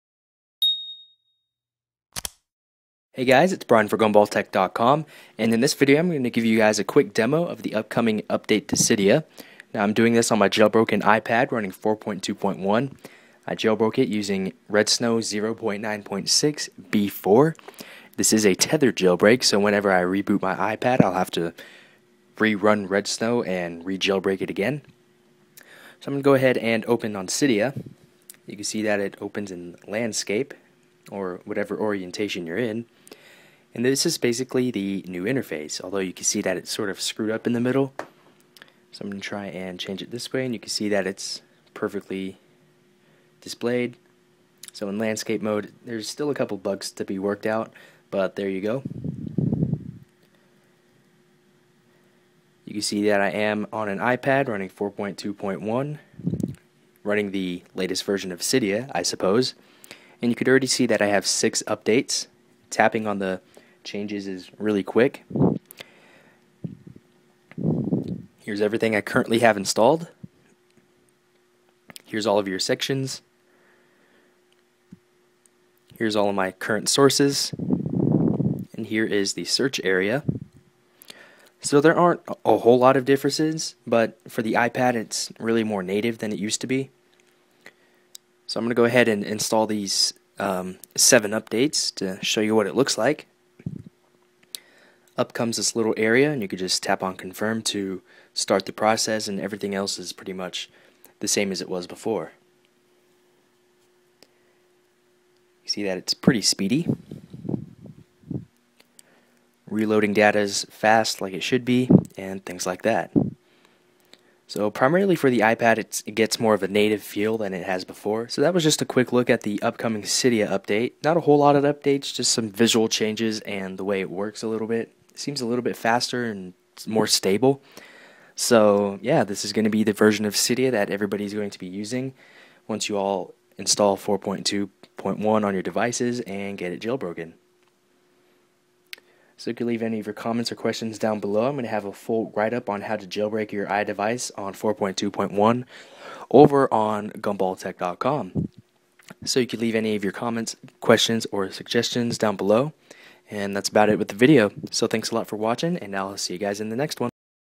New Cydia for iOS 4.2.x - A Quick Look
A quick look at the new Cydia for iOS 4.2.x. This was done with an iPad, and it's a much more native experience. I'm sure the iPhone version has been changed a little bit, but not by much.

The iPad is jailbroken (tethered, redsn0w 0.9.6b4) and is running iOS 4.2.1.